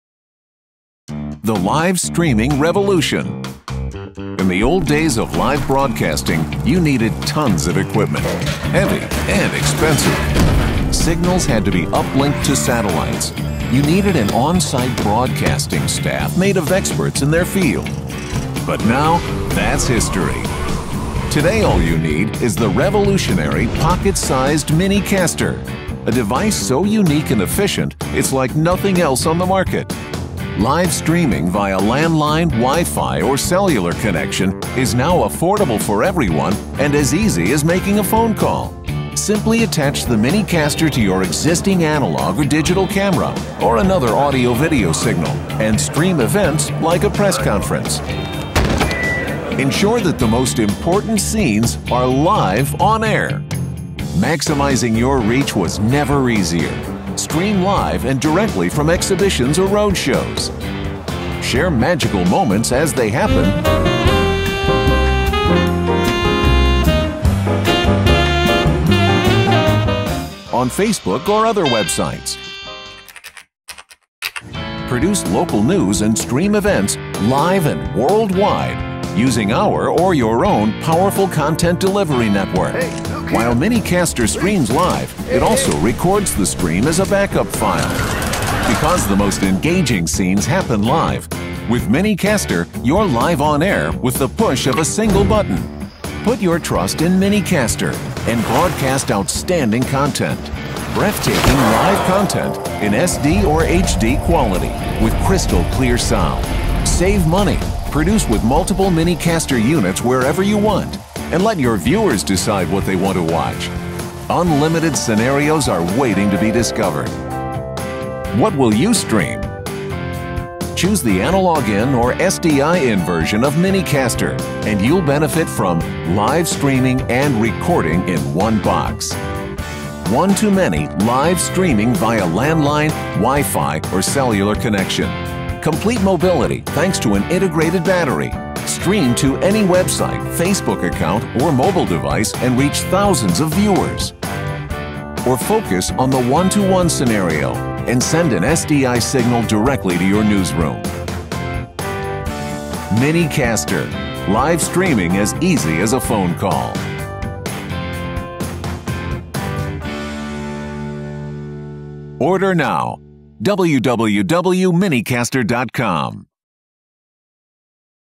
miniCASTER - live streaming as easy as a phone call
miniCASTER is the first true mobile pocket sized Live H.264 encoder - converting analog and digital audio video signals in realtime, now everybody can broadcast live not only in TV quality but in HD by the push of a button.